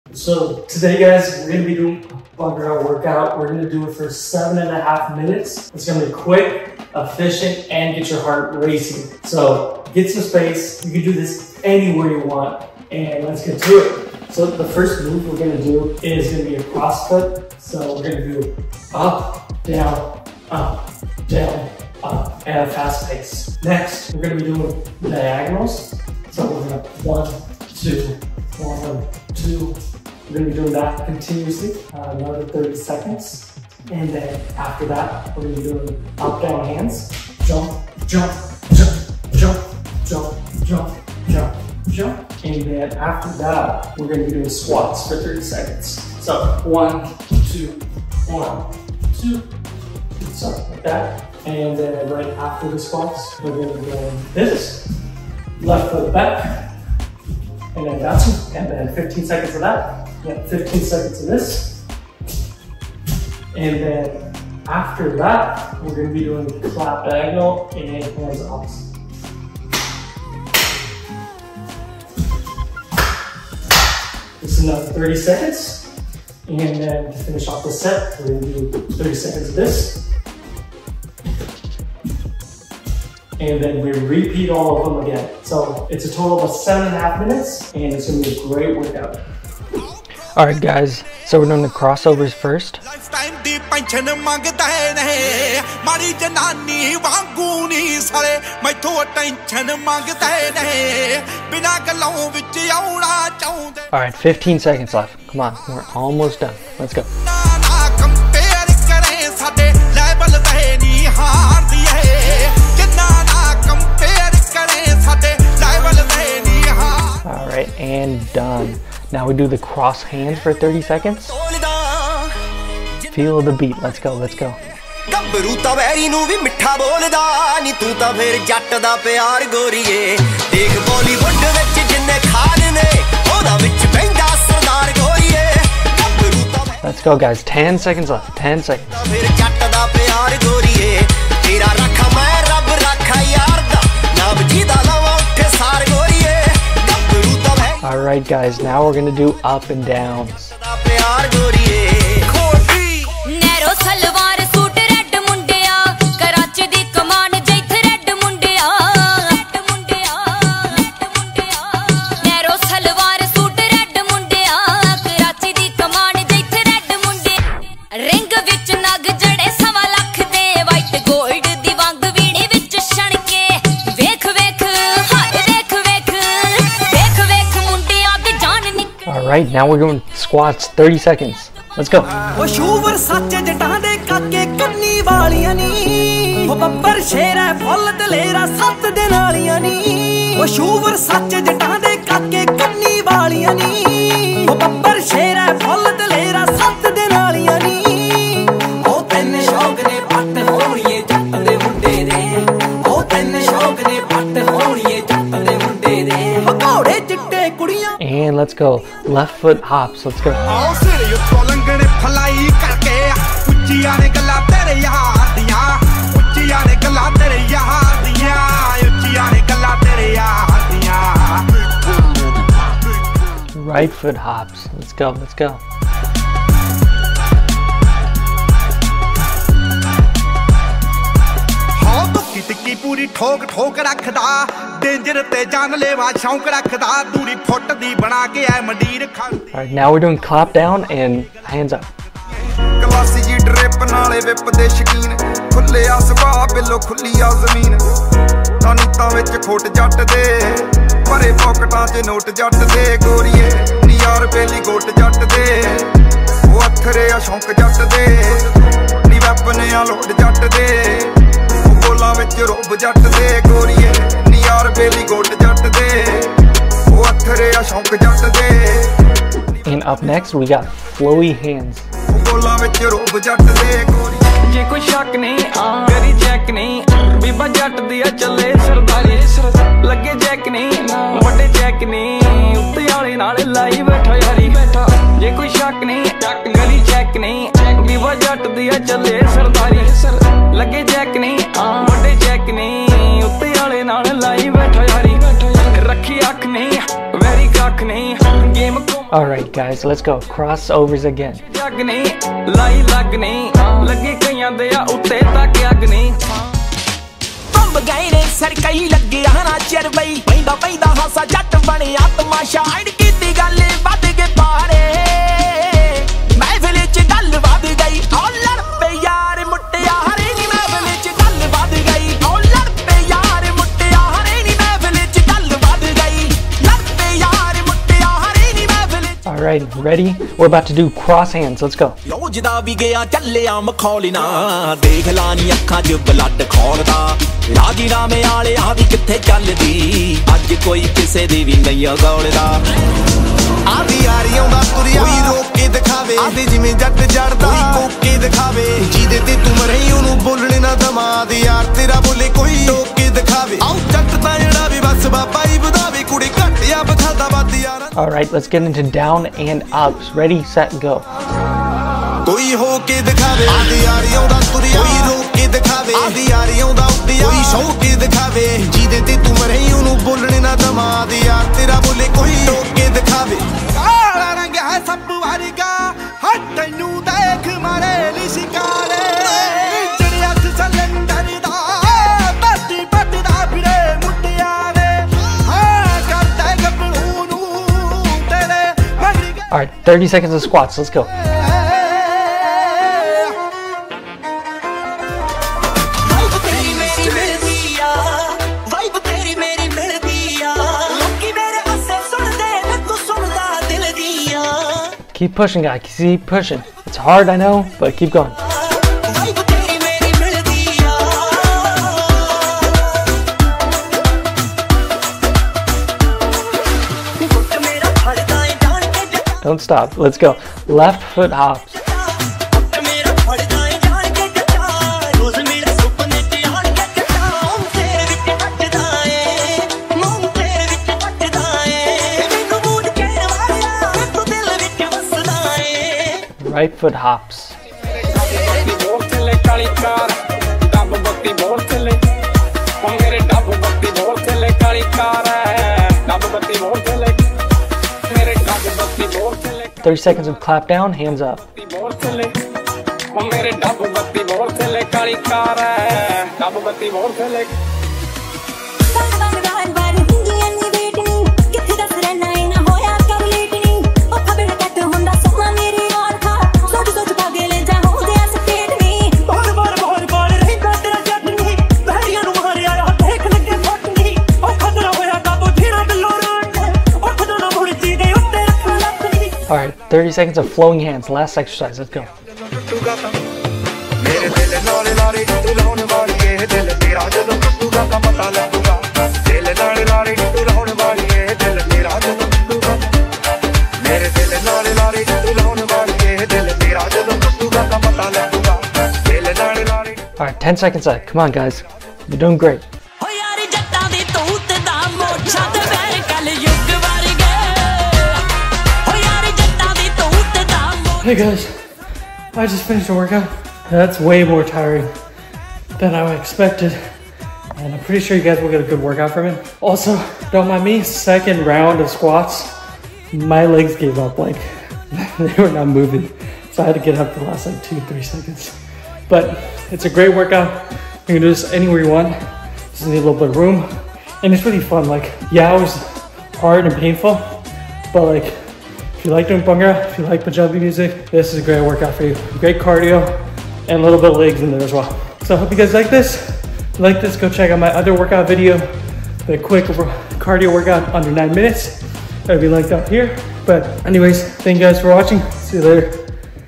7 Minute Non-Stop Bhangra Workout | Calorie Demolisher
Here is a fun and exhilariting 7 minute Bhangra workout that will get you pumped up and sweating! If your looking for a new type of cardio this is the one!

And as always stay healthy and stay happy!